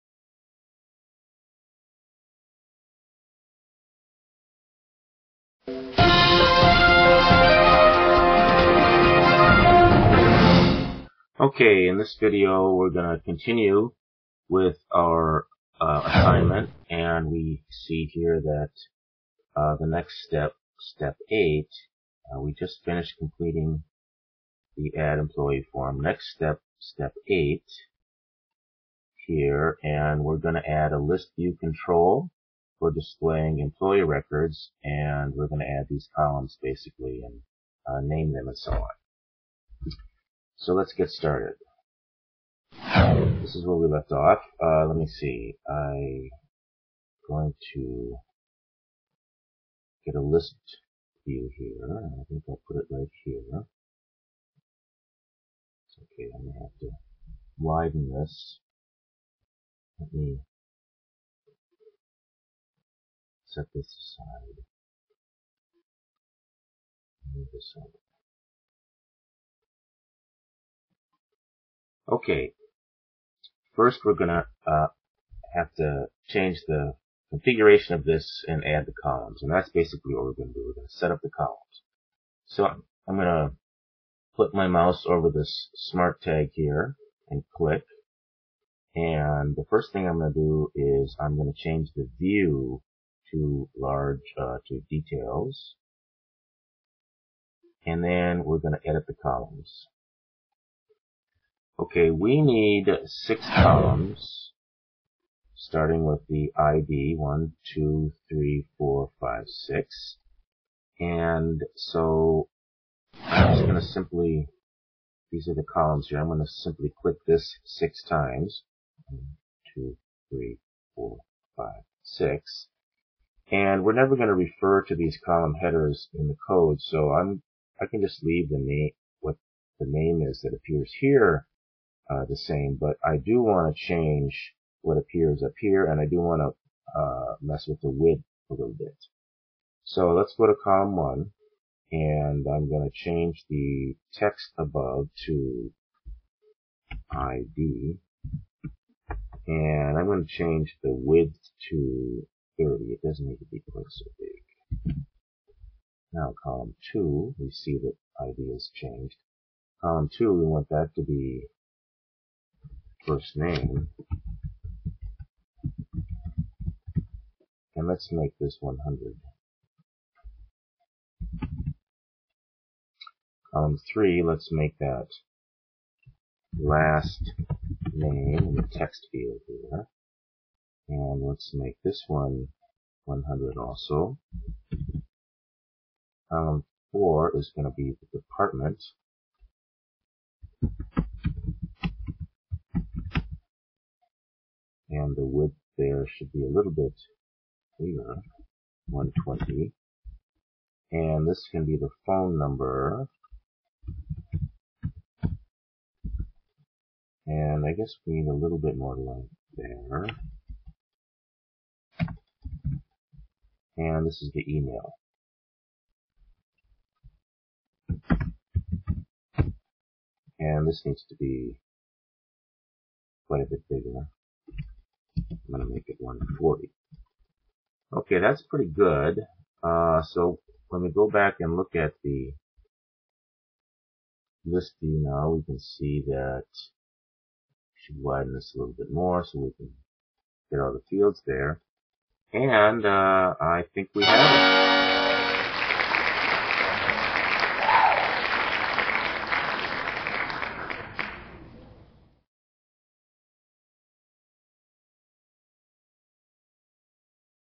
Add ListView to Visual Studio 2013 Project
Shows how to add a ListView next to the form for adding to the Employee table using Visual Studio 2013 - for ITM 354 (Part 3)